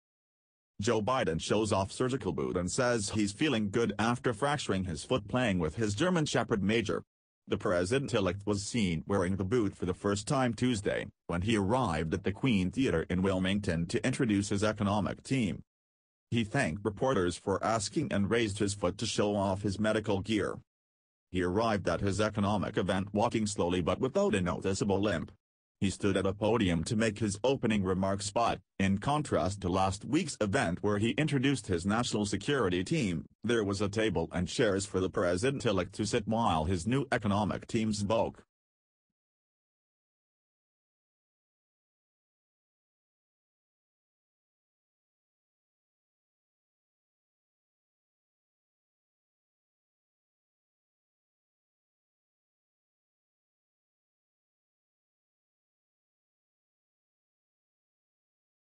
Joe Biden Shows Off Walking Boot After Foot Fracture - News 360 Tv
Joe Biden said he was doing 'good' after he fractured his foot while playing with his German Shepherd Major and showed off the black medical boot on his right foot.

The president-elect was seen wearing the boot for the first time Tuesday, when he arrived at The Queen theater in Wilmington to introduce his economic team.

He thanked reporters for asking and raised his foot to show off his medical gear.

He arrived at his economic event walking slowly but without a noticeable limp. He stood at a podium to make his opening remarks but, in contrast to last week's event where he introduced his national security team, there was a table and chairs for the president-elect to sit while his new economic team spoke.

But after standing to speak for about 15 minutes Biden limped visibly as he went to sit down after introducing his choice as Treasury Secretary, former Federal Reserve chair Janet Yellen.